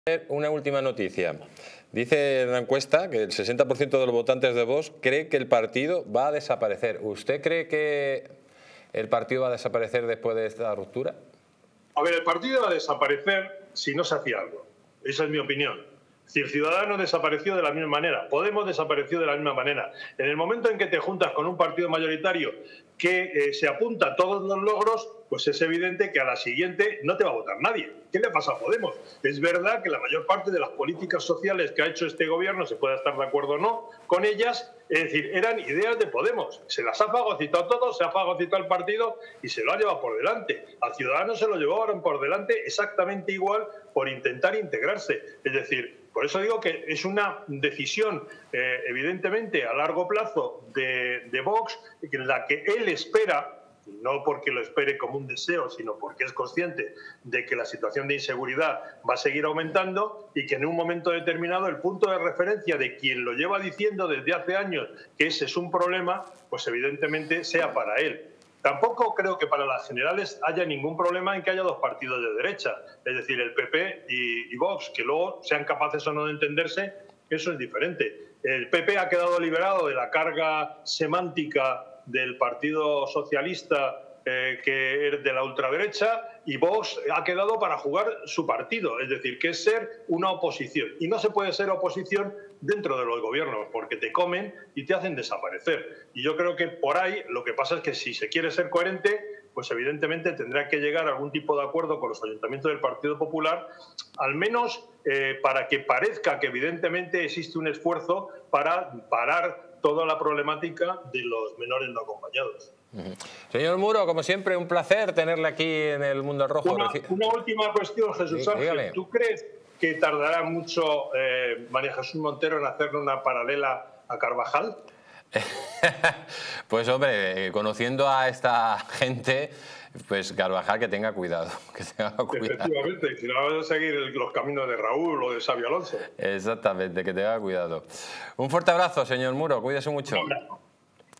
🔴El 60% de los votantes de Vox cree que el partido va a desaparecer, como ABASCAL siga ASÍ🔴
En esta transmisión en vivo, analizaremos por qué el 60% de los votantes de Vox creen que el partido va a desaparecer. ¡No te lo pierdas!